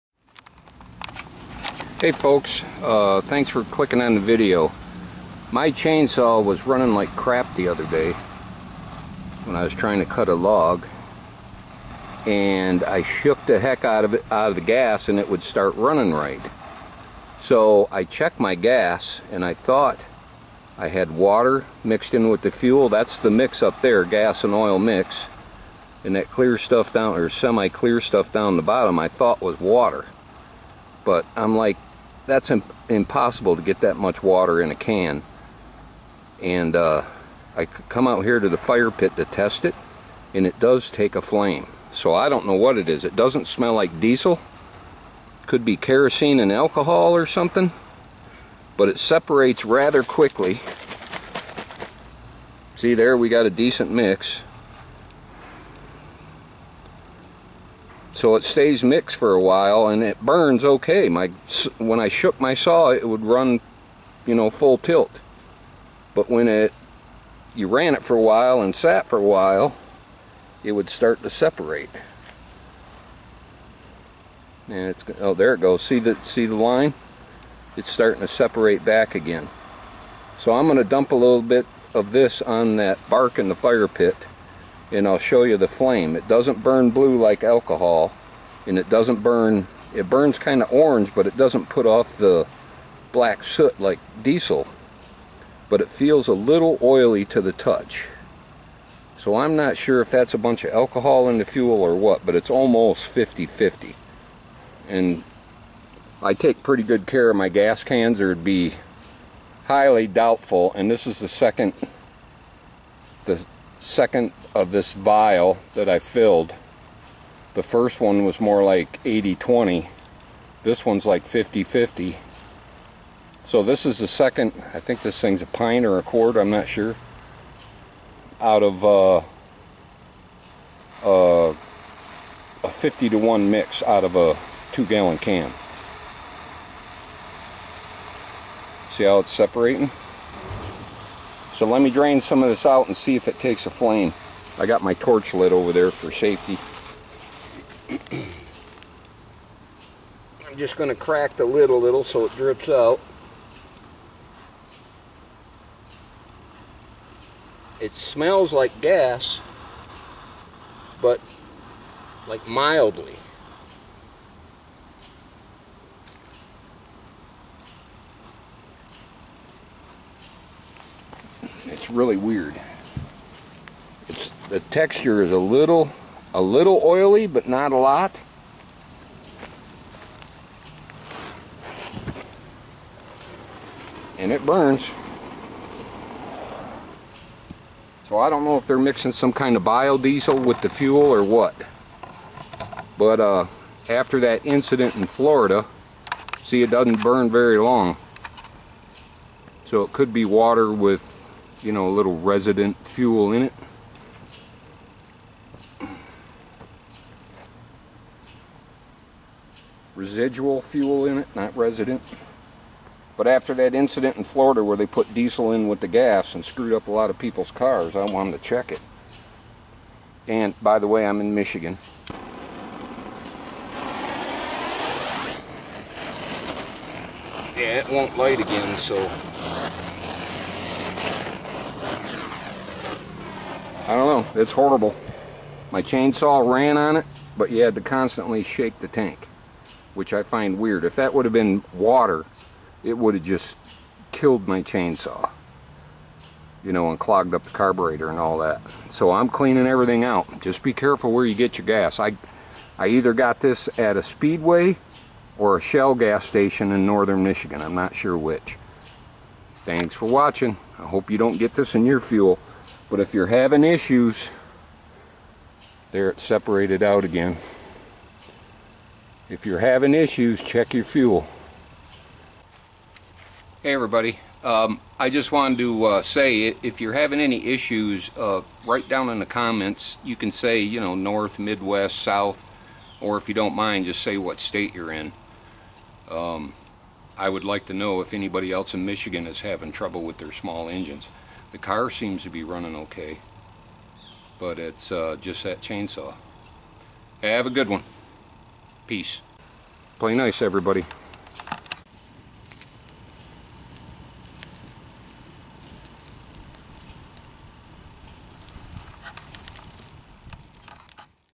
Fuel problems in Michigan
I have some kind of contaminate in my Gas . Anyone else having issues ?